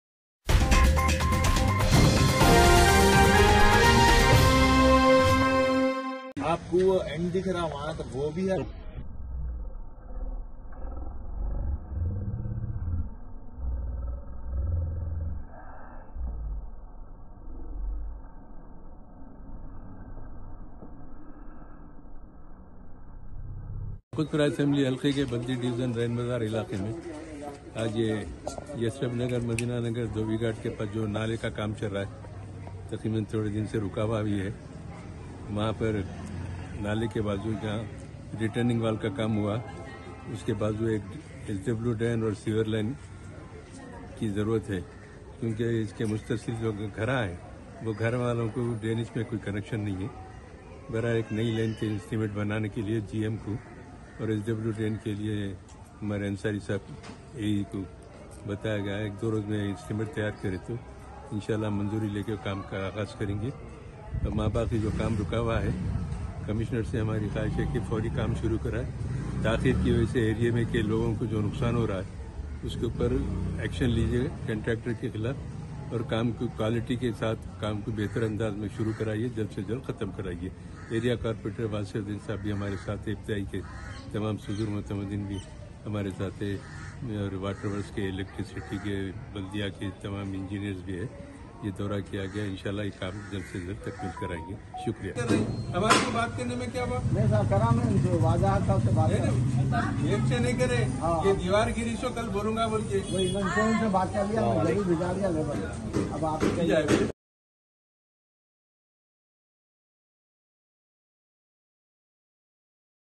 Barish Ka Silsila Jari Yaqut Pura Mla Ahmed Pasha Quadri Ne Kiya Rain Bazar Division Ka Doura..
Naqeeb E Millat Aalijanab Alhaj Barrister Asaduddin Owaisi Sahab (President AIMIM And MP Hyderabad) Ki Hidayat Per Janab Syed Ahmed Pasha Quadri Sahab (General Secretary AIMIM And MLA Yakutpura) Ne Janab Mohammed Wassay Uddin Sahab ( Corporator Rein Bazar Division) Ke Saath Yasrab Nagar, Madina Nagar Ka Daura Kiya Aur Muqtalif Baldi Masayel Ka Jayeza Liya.

Is Mouqe Per Ibtedai Majlis Ke Zimmedaran Bhi Maujood The